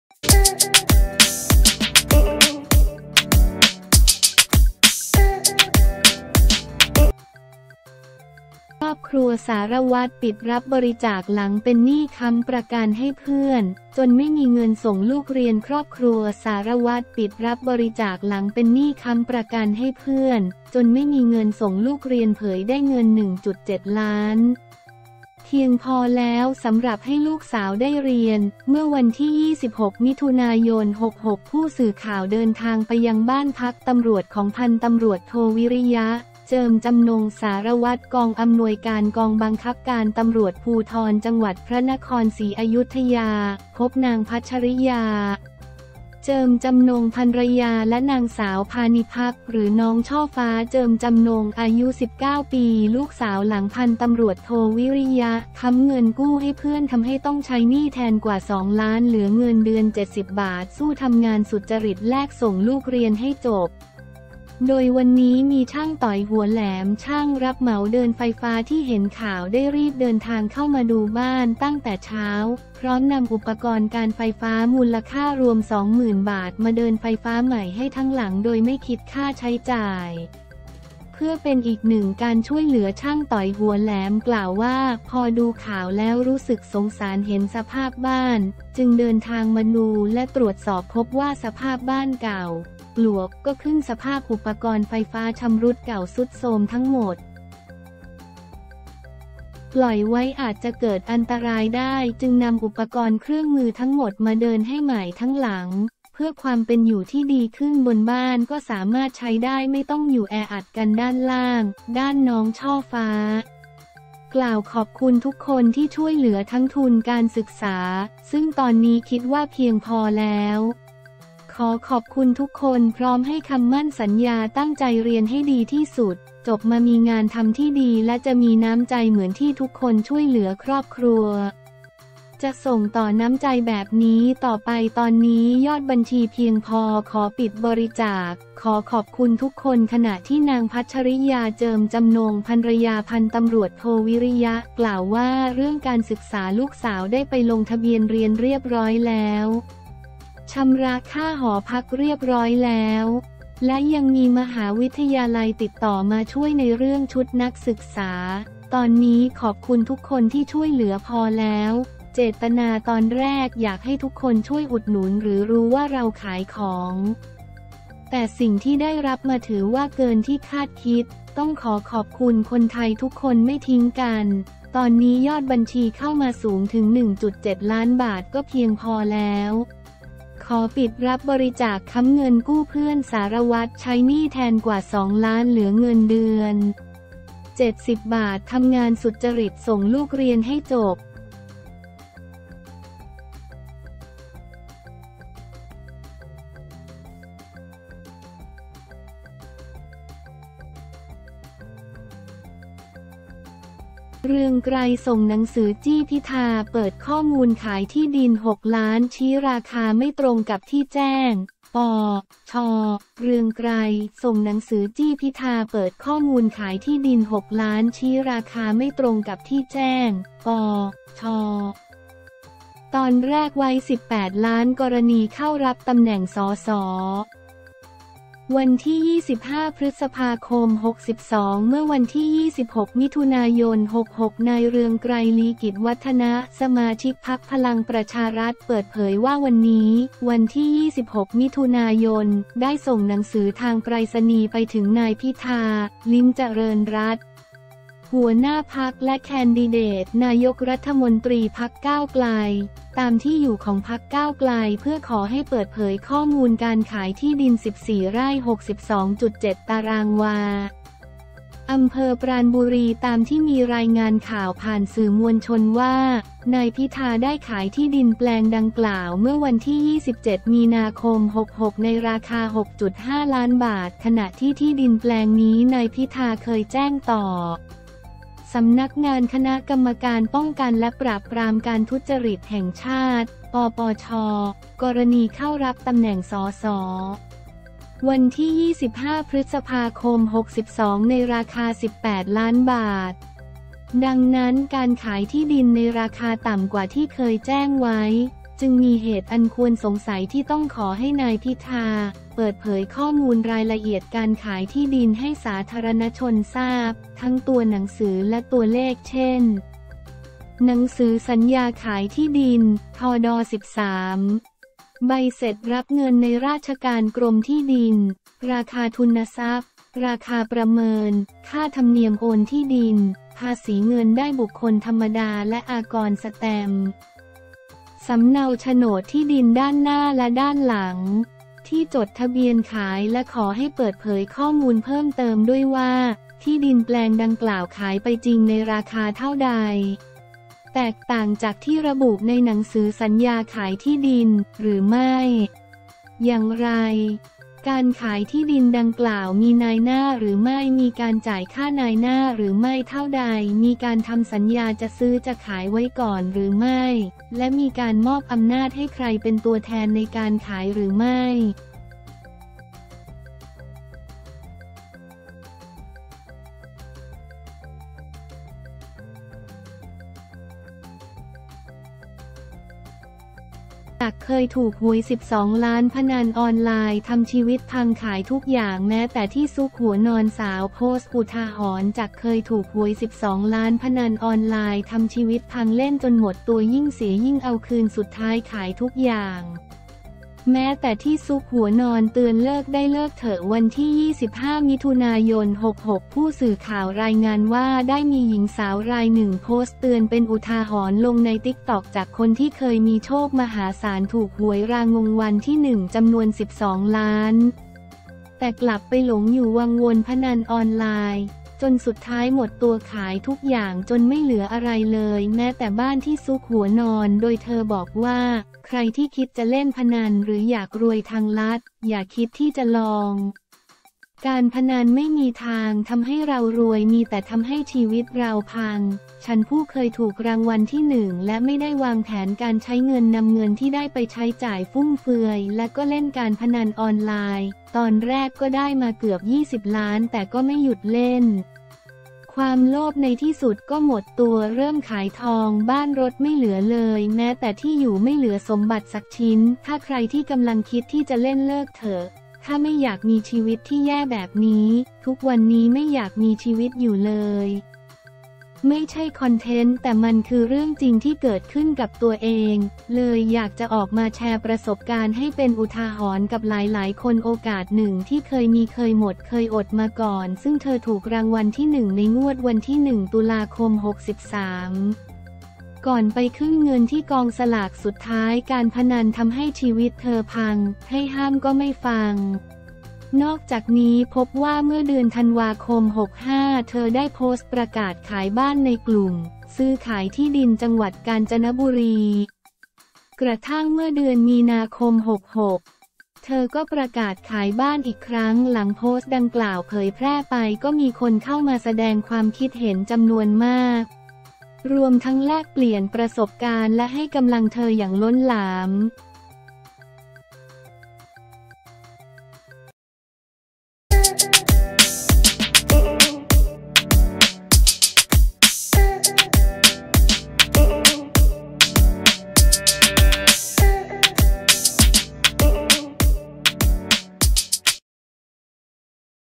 ครอบครัวสารวัตรปิดรับบริจาค หลังเป็นหนี้ค้ำประกันให้เพื่อน จนไม่มีเงินส่งลูกเรียน
ครอบครัวสารวัตรปิดรับบริจาค หลังเป็นหนี้ค้ำประกันให้เพื่อน จนไม่มีเงินส่งลูกเรียน เผยได้เงิน 1.7 ล้าน เพียงพอแล้วสำหรับให้ลูกสาวได้เรียน

เมื่อวันที่ 26 มิ.ย.66 ผู้สื่อข่าวเดินทางไปยังบ้านพักตำรวจของพ.ต.ท.วิริยะ เจิมจำนงค์ สารวัตรกองอำนวยการ กองบังคับการตำรวจภูธรจังหวัดพระนครศรีอยุธยา พบนางพัชริยา เจิมจำนงค์ ภรรยา และน.ส.พาณิภัค หรือน้องช่อฟ้า เจิมจำนง อายุ 19 ปี ลูกสาว หลังพ.ต.ท.วิริยะ ค้ำเงินกู้ให้เพื่อน ทำให้ต้องใช้หนี้แทนกว่า 2 ล้าน เหลือเงินเดือน 70 บาท สู้ทำงานสุจริตแลก ส่งลูกเรียนให้จบ
โดยวันนี้มีช่างต๋อย หัวแหลม ช่างรับเหมาเดินไฟฟ้า ที่เห็นข่าวได้รีบเดินทางเข้ามาดูบ้านตั้งแต่เช้า พร้อมนำอุปกรณ์การไฟฟ้า มูลค่ารวม 20,000 บาท มาเดินไฟฟ้าใหม่ให้ทั้งหลัง โดยไม่คิดค่าใช้จ่าย เพื่อเป็นอีกหนึ่งการช่วยเหลือ
ช่างต๋อย หัวแหลม กล่าวว่า พอดูข่าวแล้วรู้สึกสงสารเห็นสภาพบ้าน จึงเดินทางมาดูและตรวจสอบพบว่าสภาพบ้านเก่า ปลวกก็ขึ้นสภาพอุปกรณ์ไฟฟ้าชำรุดเก่าทรุดโทรมทั้งหมด ปล่อยไว้อาจจะเกิดอันตรายได้ จึงนำอุปกรณ์เครื่องมือทั้งหมดมาเดินให้ใหม่ทั้งหลัง เพื่อความเป็นอยู่ที่ดีขึ้น บนบ้านก็สามารถใช้ได้ไม่ต้องอยู่แออัดกันด้านล่าง
ด้านน้องช่อฟ้า กล่าวขอบคุณทุกคนที่ช่วยเหลือทั้งทุนการศึกษา ซึ่งตอนนี้คิดว่าเพียงพอแล้ว ขอขอบคุณทุกคนพร้อมให้คำมั่นสัญญาตั้งใจเรียนให้ดีที่สุด จบมามีงานทำที่ดี และจะมีน้ำใจเหมือนที่ทุกคนช่วยเหลือครอบครัว จะส่งต่อน้ำใจแบบนี้ต่อไป ตอนนี้ยอดบัญชีเพียงพอขอปิดบริจาค ขอขอบคุณทุกคน

ขณะที่นางพัชริยา เจิมจำนงค์ ภรรยาพ.ต.ท.วิริยะ กล่าวว่า เรื่องการศึกษาลูกสาวได้ไปลงทะเบียนเรียนเรียบร้อยแล้ว ชำระค่าหอพักเรียบร้อยแล้ว และยังมีมหาวิทยาลัยติดต่อมาช่วยในเรื่องชุดนักศึกษา ตอนนี้ขอบคุณทุกคนที่ช่วยเหลือพอแล้ว เจตนาตอนแรกอยากให้ทุกคนช่วยอุดหนุนหรือรู้ว่าเราขายของ แต่สิ่งที่ได้รับมาถือว่าเกินที่คาดคิด ต้องขอขอบคุณคนไทยทุกคนไม่ทิ้งกัน ตอนนี้ยอดบัญชีเข้ามาสูงถึง 1.7 ล้านบาท ก็เพียงพอแล้ว ขอปิดรับบริจาค

ค้ำเงินกู้เพื่อน สารวัตรใช้หนี้แทนกว่า 2 ล้าน เหลือเงินเดือน 70 บาท ทำงานสุจริต ส่งลูกเรียนให้จบ